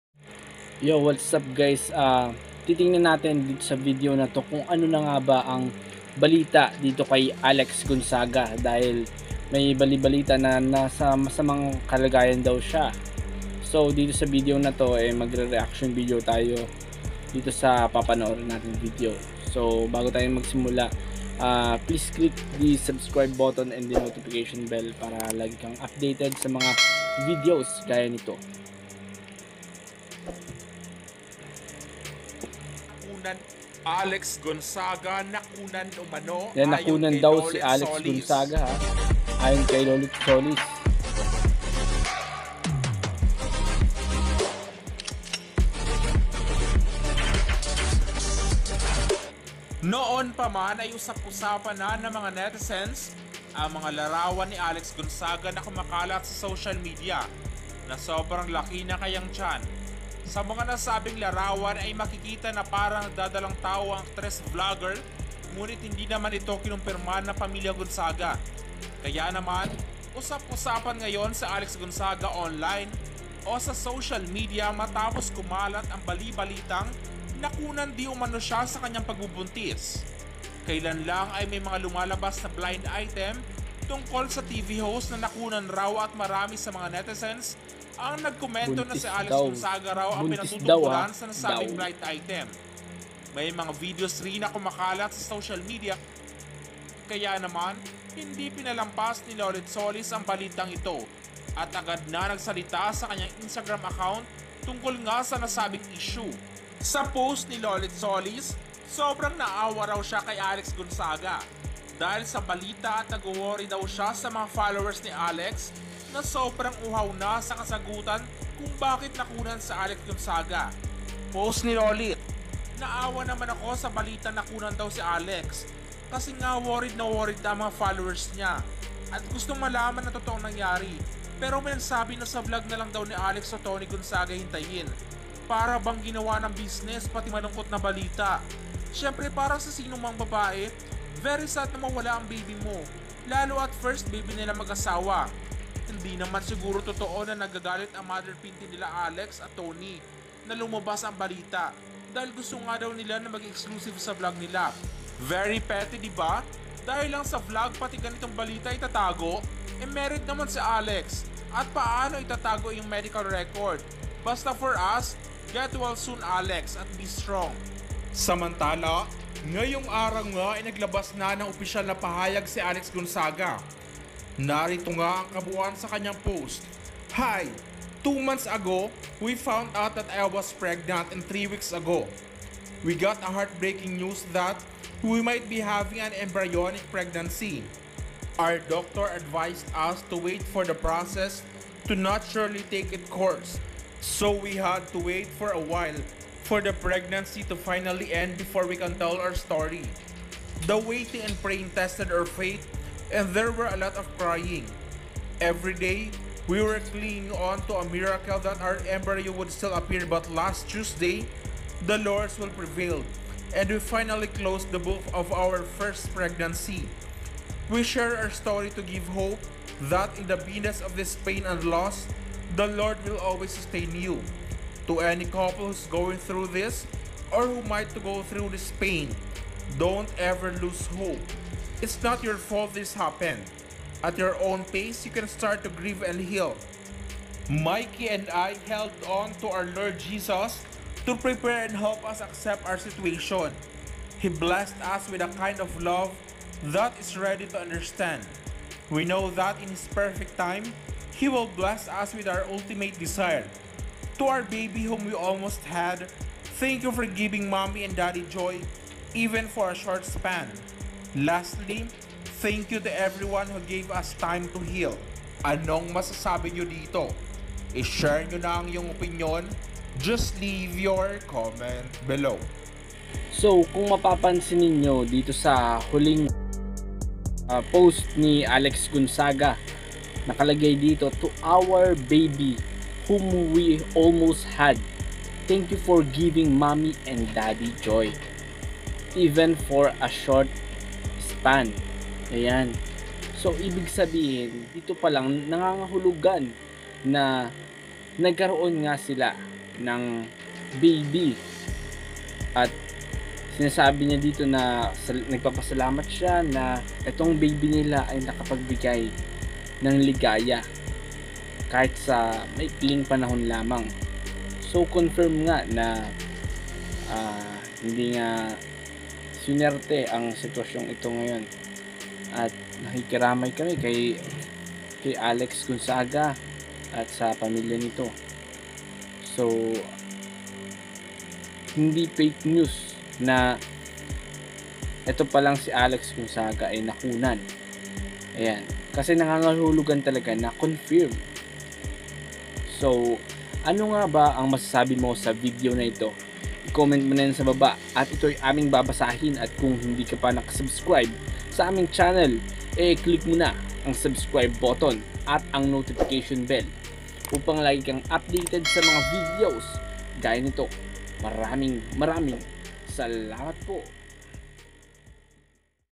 ALEX GONZAGA NAGBUNTIS PALA PERO NAWALA ANG DINADALA | FAKE NEWS NGA BA??
ALEZ GONZAGA BUNTIS
nagdadalang tao
nalaglag
baby
mommy pinty
lolita Solis
trending
blogger
youtuber
Alex GONZAGA updates
Avee Ola